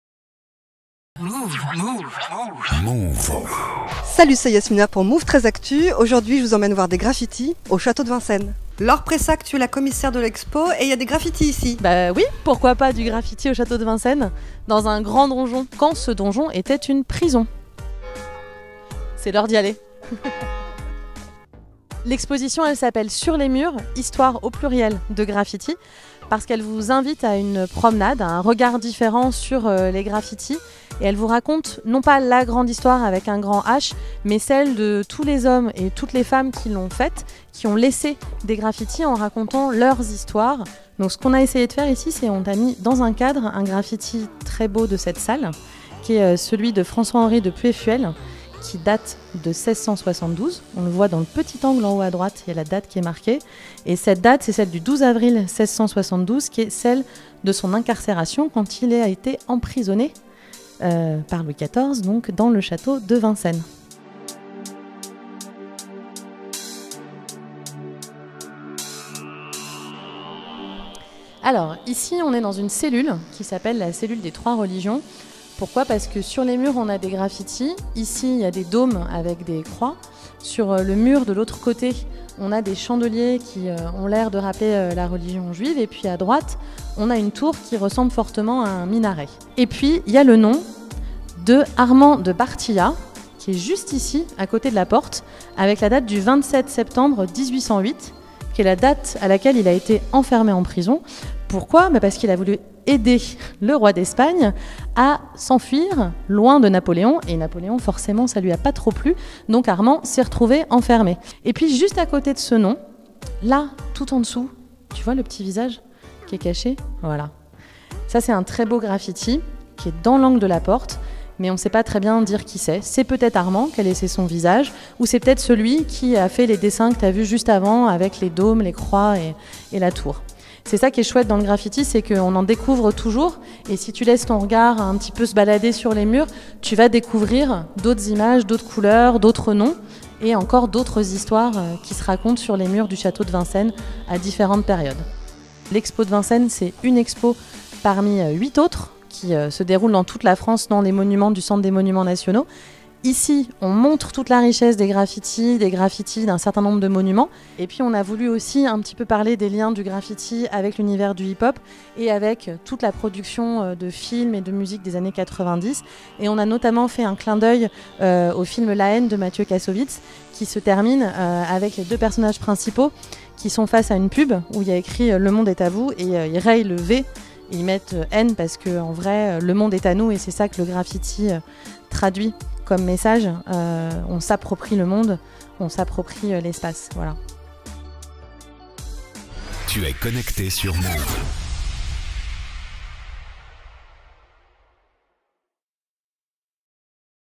Des graffitis sur les murs du château de Vincennes
Le Centre des Monuments Nationaux présente l'expo "Sur les murs, histoire (s) de graffiti " au Château de Vincennes montre des graffiti historiques… dont certains datent du 16eme siècle… Yasmina y était.